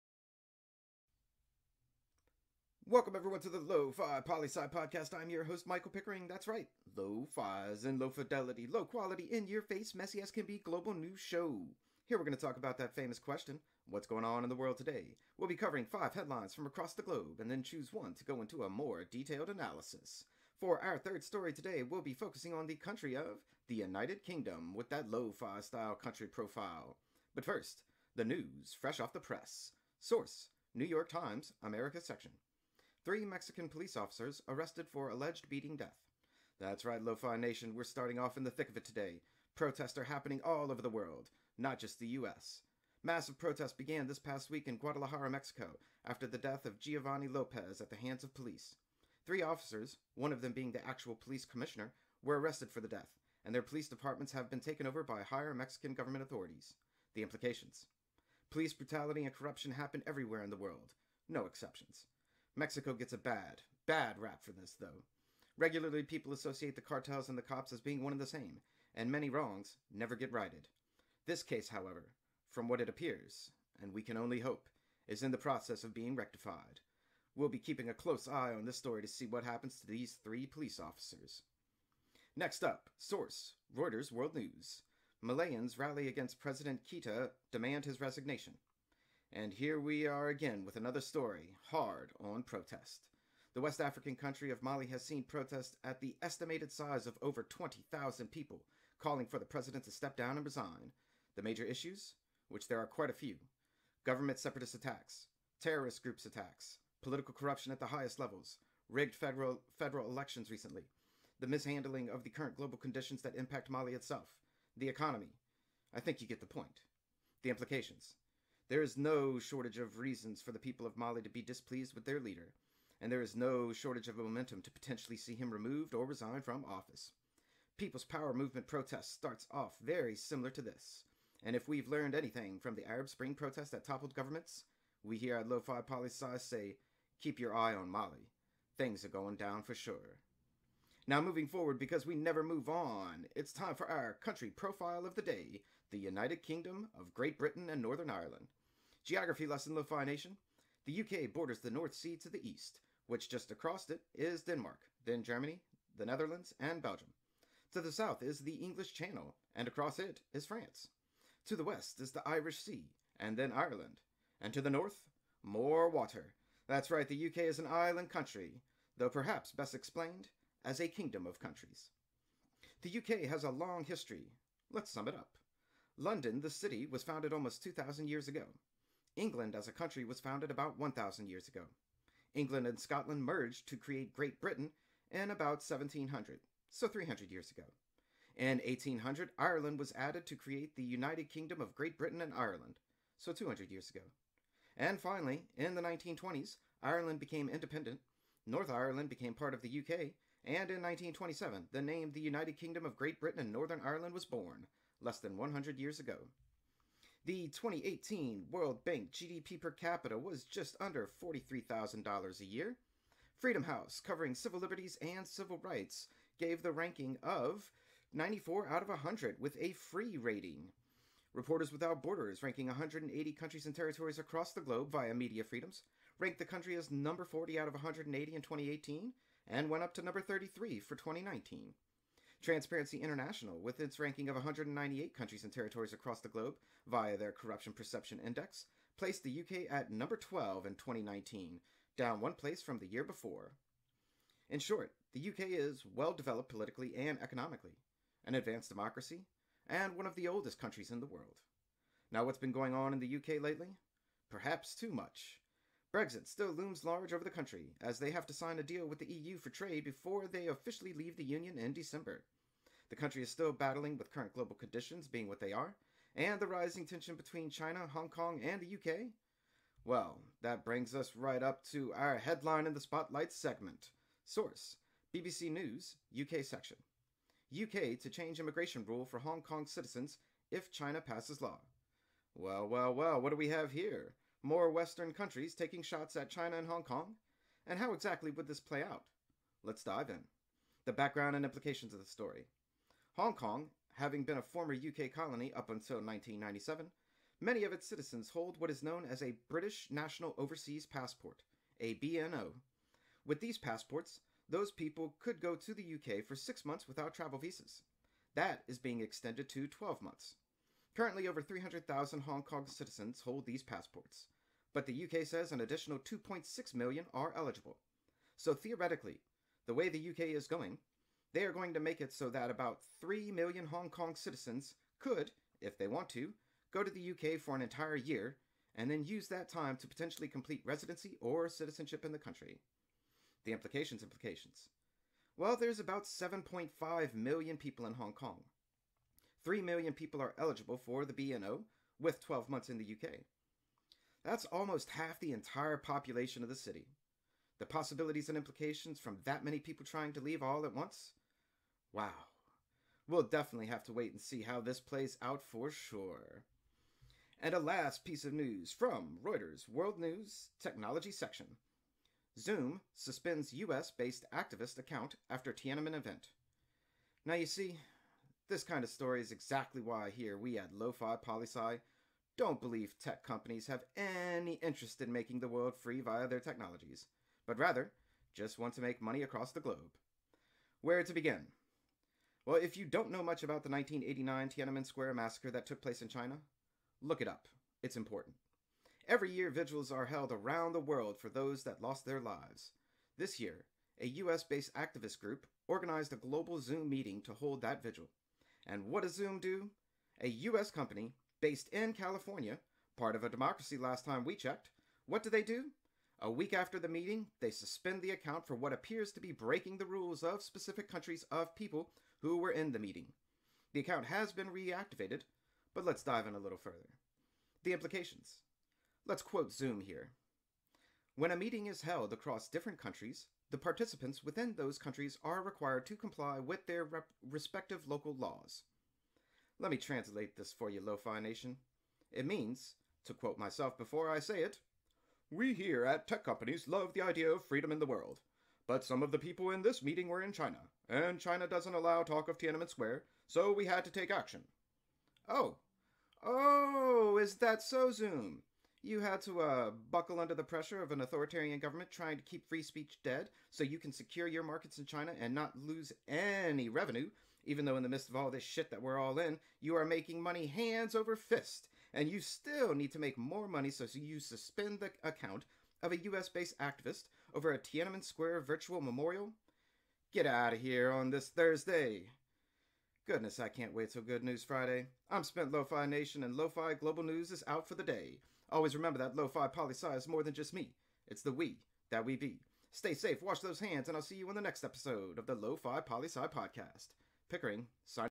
“lofi-Global News”
Always remember that Lofi Poli Sci is more than just me, it’s the “we”, that we be.

Countries/Topics Covered: Mali, Mexico, UK, Hong Kong-UK Citizenship, STUPID Zoom-China